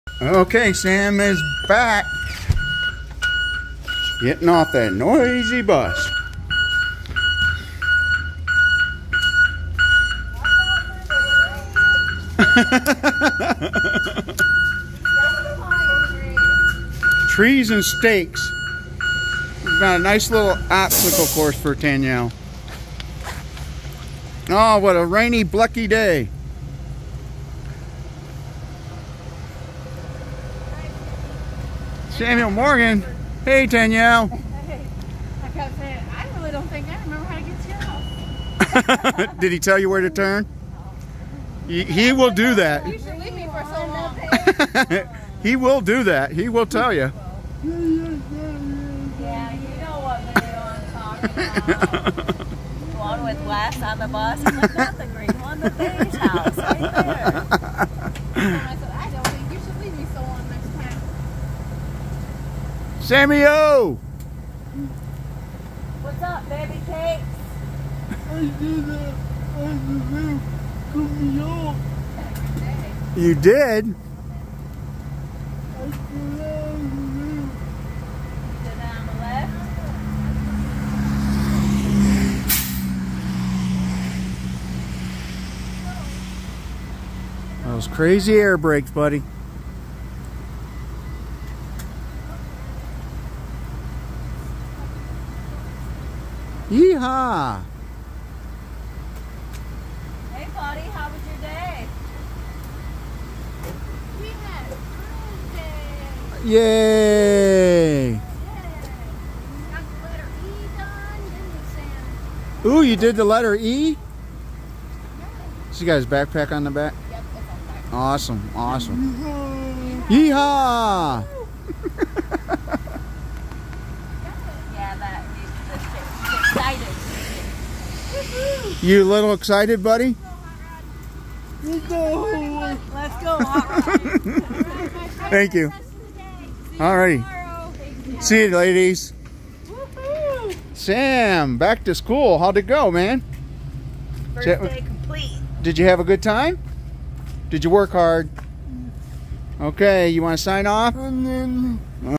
Sam is getting home from school on the school bus 1.3.23
Sam's 1st day off the bus after school.
Rachel helps him get of the bus.